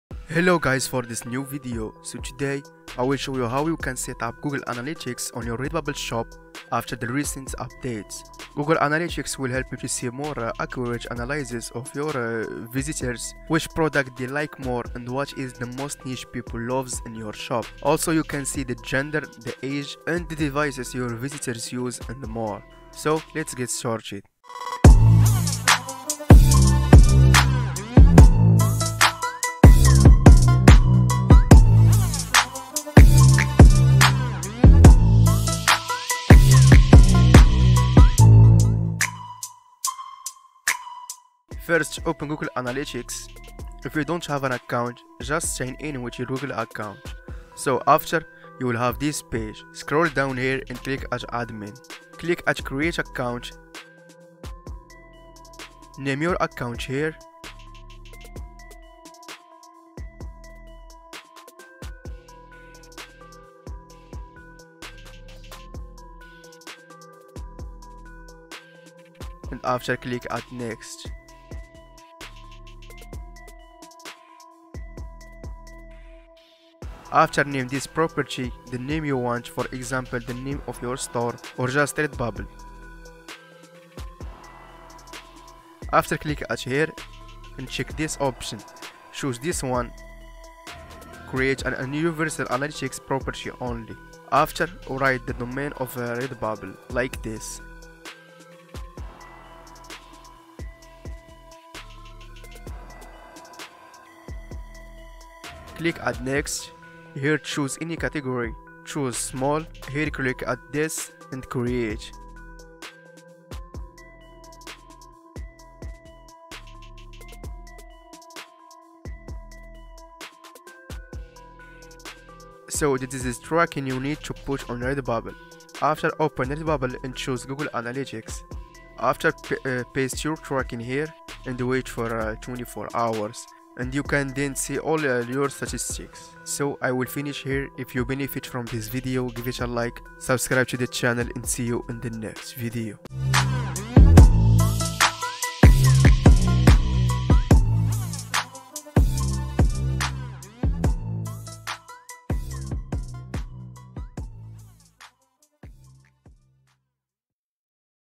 Setup Google Analytics For Redbubble In Just 1 Minute
In This Video I Will Show How You Can Setup Google Analytics on Your Redbubble Shop With Easy And Fast Steps